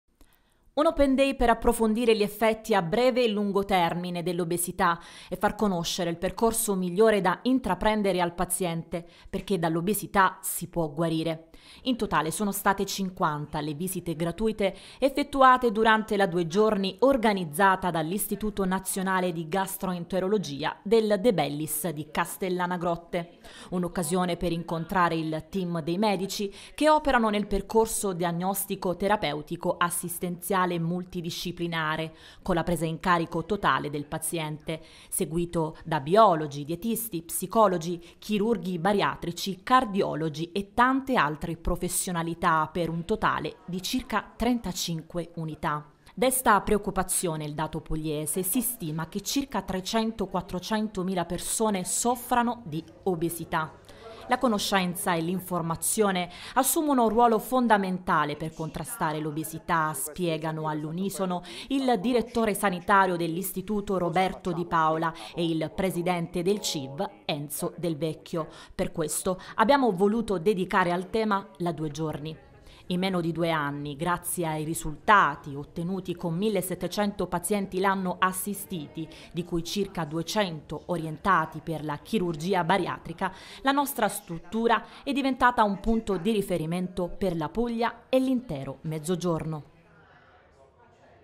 Prevenzione a cura dell'obesità, visite al “De Bellis” di Castellana Grotte
Una due giorni per sottolineare l'importanza della prevenzione e dell’informazione per combattere l’obesità, responsabile di molte patologie gravi. Al “De Bellis” di Castellana Grotte visite gratuite per i cittadini.